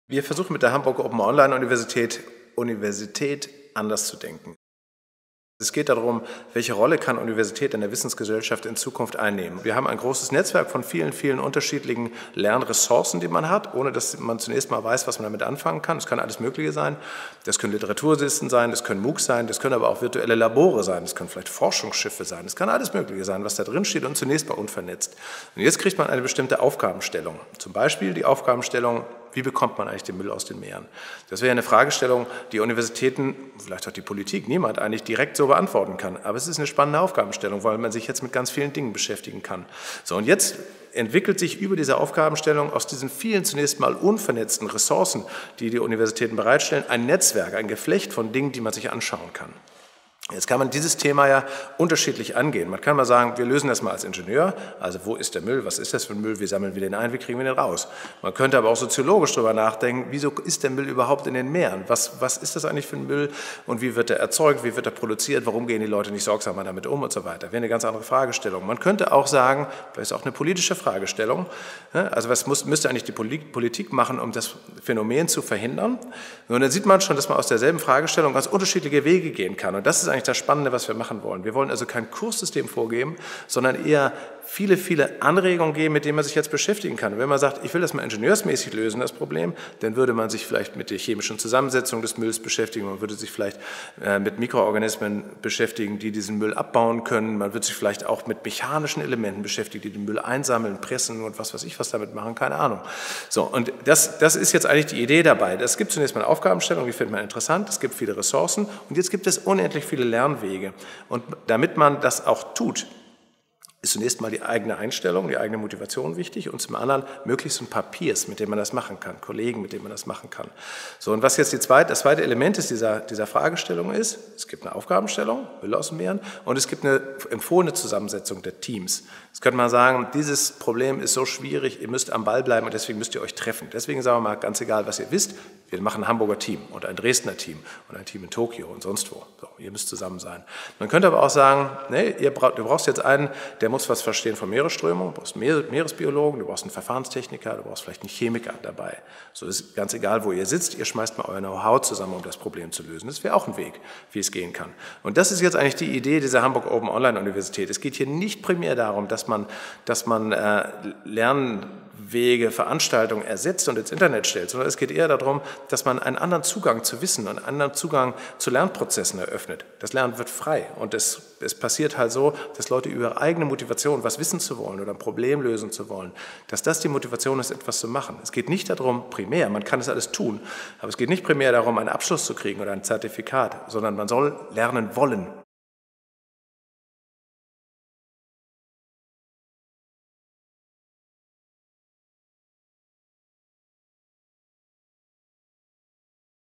Sönke Knutzen: Lernen wird frei
Wie will die derzeit im Entstehen begriffene Hamburg Open Online Universität Bildung neu denken? Sönke Knutzen (TU Hamburg-Harburg), der die Expertengruppe Portalkonzept und Technik leitet, zeigt die Perspektiven auf: Ein starres Kurssystem will die neue Internet-Hochschule nicht vorgeben.

Produktion: Corina Niebuhr, Webclip Medien Berlin
für den Bildungskanal des Stifterverbandes: